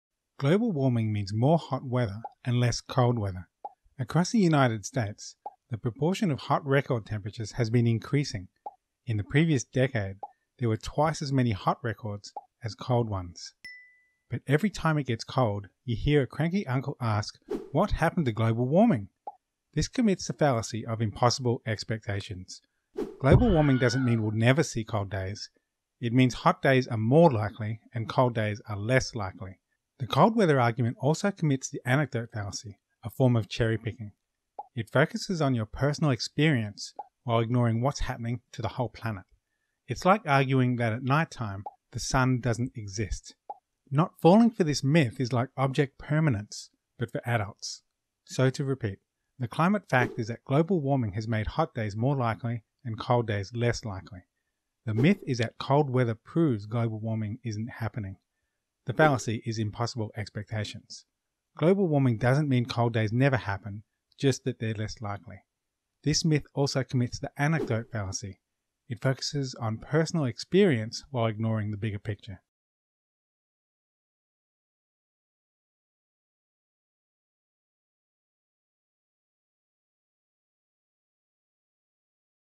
Debunking Cranky Uncle on cold weather
A debunking of the "cold weather proves global warming doesn't exist" myth, using cartoons from the Cranky Uncle vs. Climate Change book: